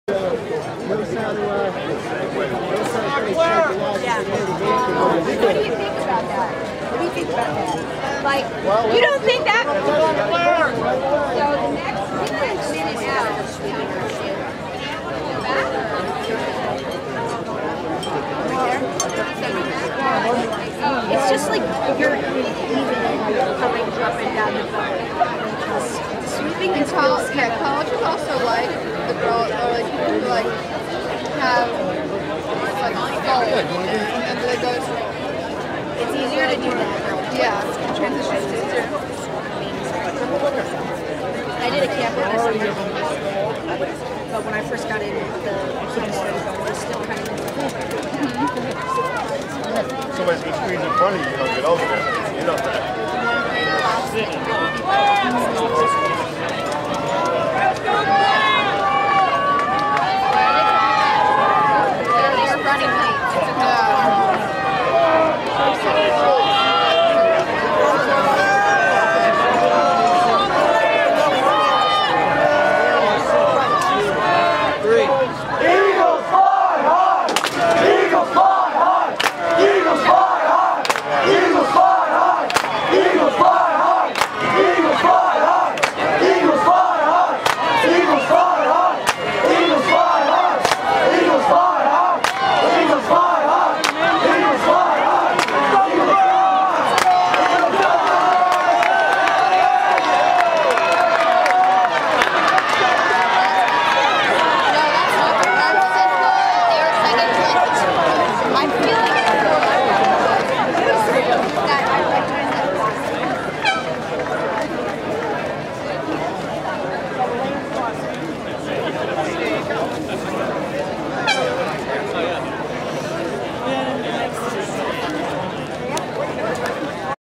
2012 SRAA Nationals Regatta - Mens Freshman 8 - Heat - 05/25/2012
TC Williams Crew competing at the Scholastic Rowing Association of America Nationals Regatta - Camden, NJ